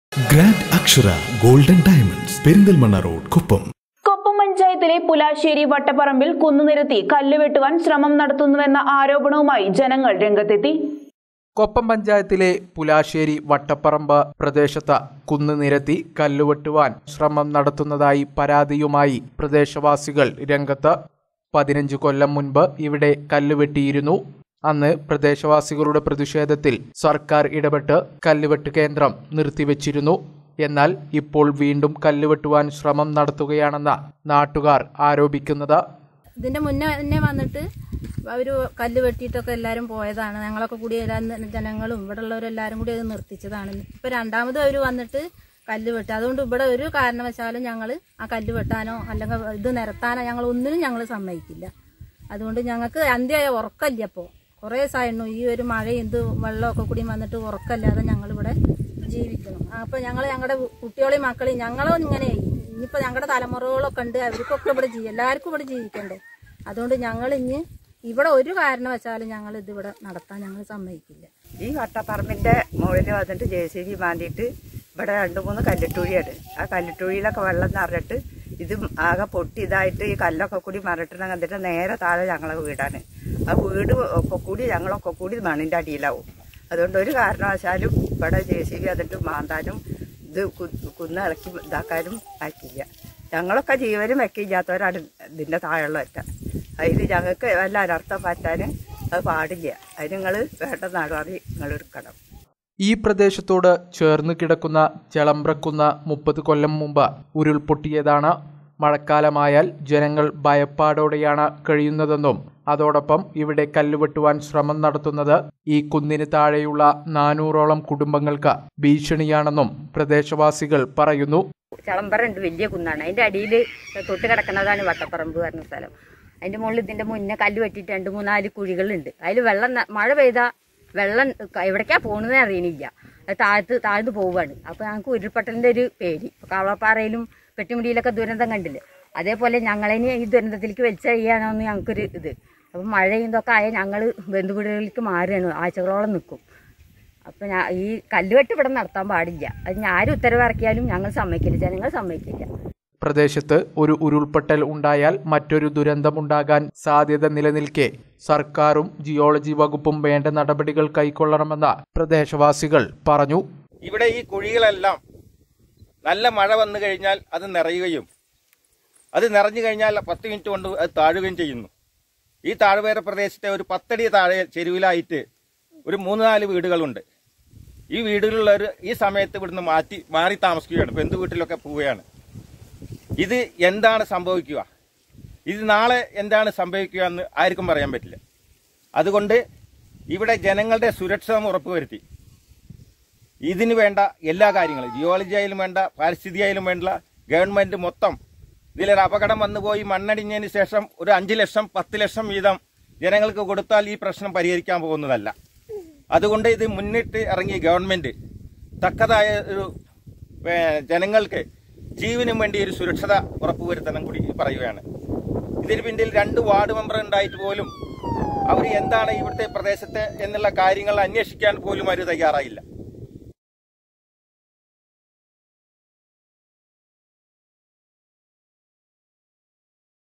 പുലാശ്ശേരി വട്ടപ്പറമ്പിൽ കുന്നു നിരത്തി കല്ലു വെട്ടുവാൻ ശ്രമം നടത്തുന്നുവെന്ന ആരോപണവുമായി ജനങ്ങൾ
കൊപ്പം പഞ്ചായത്തിലെ പുലാശ്ശേരി വട്ടപ്പറമ്പിൽ കുന്നു നിരത്തി കല്ലു വെട്ടുവാൻ ശ്രമം നടത്തുന്നുവെന്ന ആരോപണവുമായി ജനങ്ങൾ രംഗത്തെത്തി